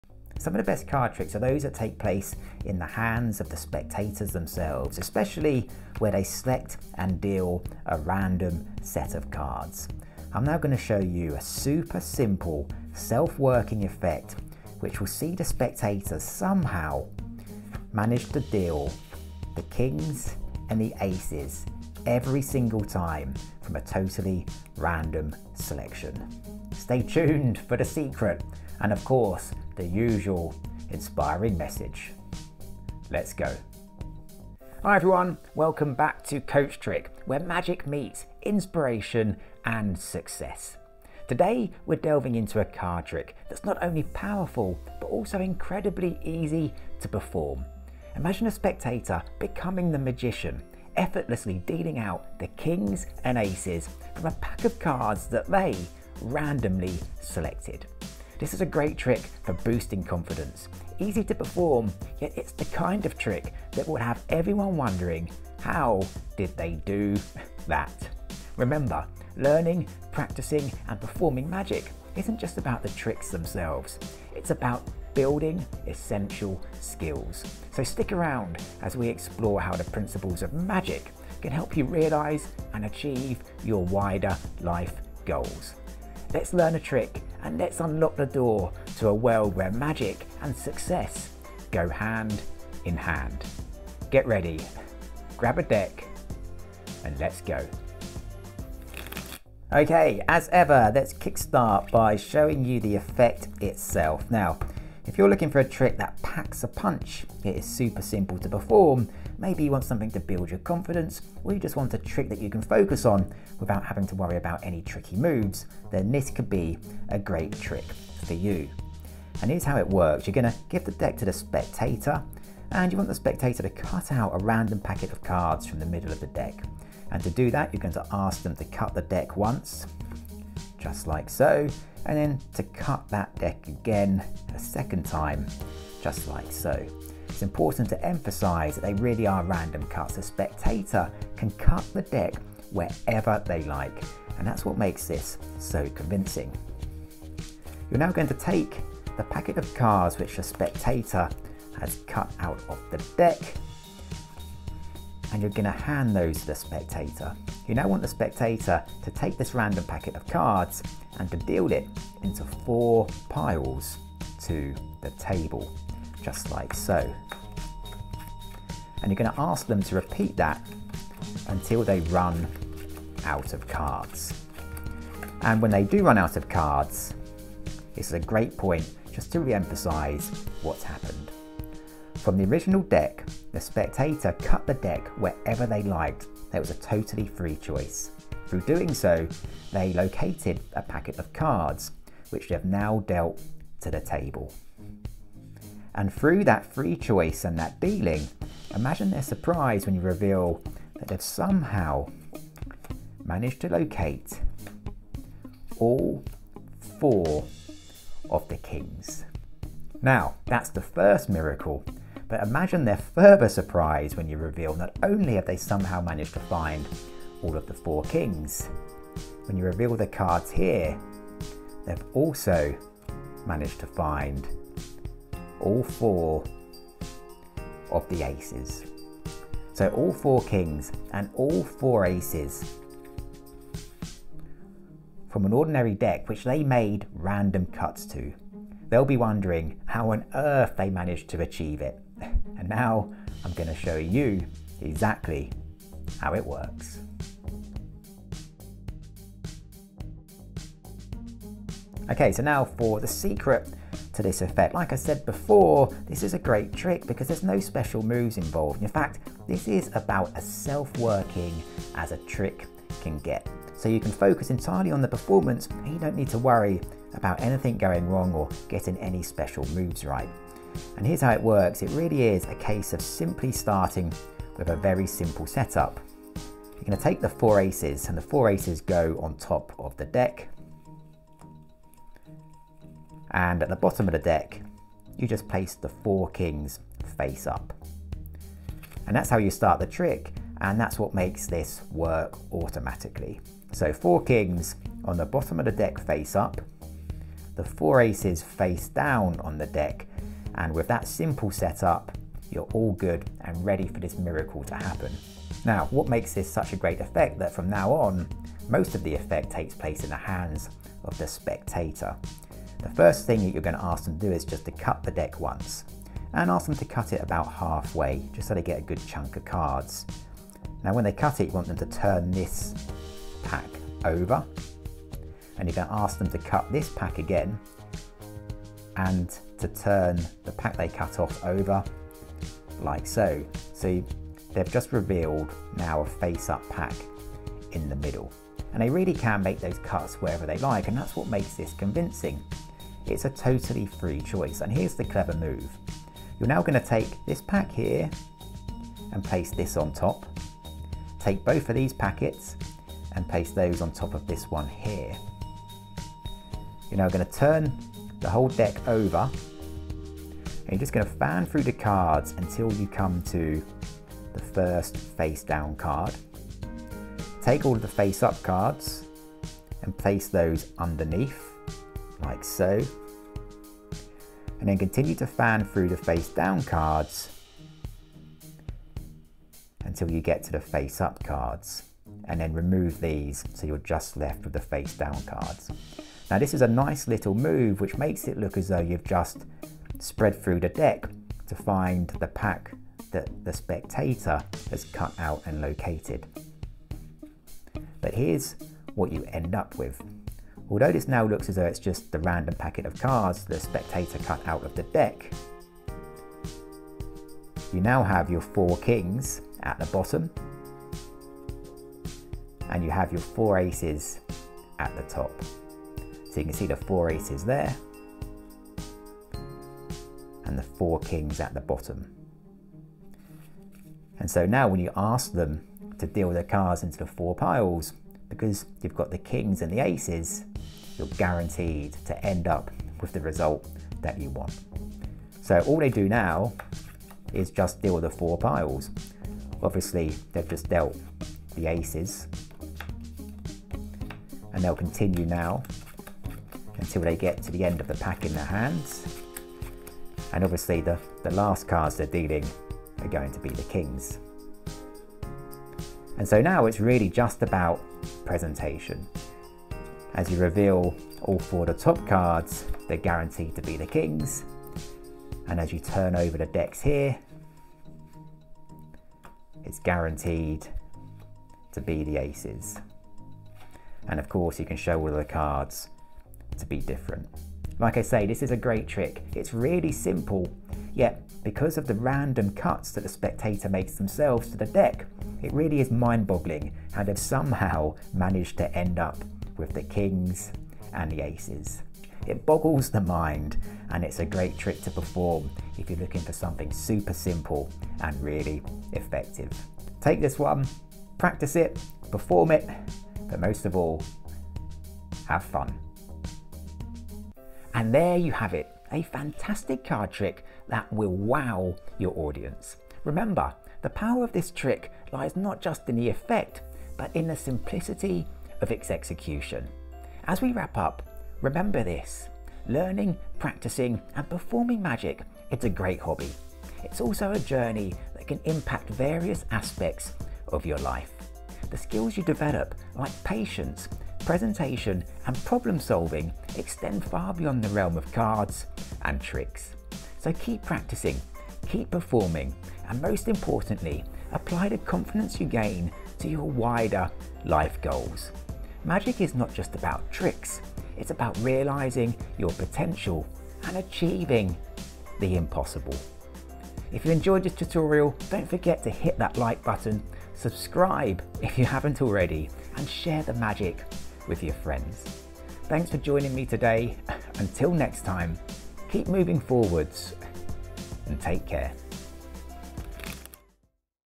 Kings and Aces Unleashed: Easy Card Trick Tutorial!
In this eye-opening tutorial, we share a self-working card trick that packs a punch. Get ready for an effect where your spectator becomes the star, effortlessly dealing out the Kings and Aces from a random selection of cards.

Here's the exciting part - this trick is so easy to perform that anyone can do it, making it perfect for beginners and seasoned magicians alike. It's self-working magic at its finest, requiring no complex moves but delivering a powerful impact that'll leave your audience amazed.

But this tutorial isn't just about the trick itself. We'll also explore the fascinating link between learning magic and building confidence to achieve your goals in life. Magic isn't just about the sleights and tricks; it's a journey that can boost your self-assurance, creativity, and the belief that you can achieve the seemingly impossible.

So, grab a deck and let's dive into this exciting self-working card trick tutorial!

Credits:

Original trick: Double-Dealing by Karl Fulves.